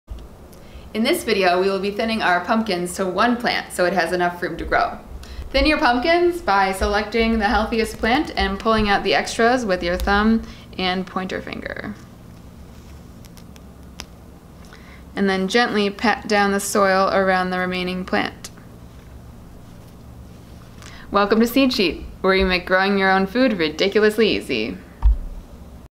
How to Thin Pumpkins
Thin Pumpkin plants to give them enough room to grow and produce lots of fruit.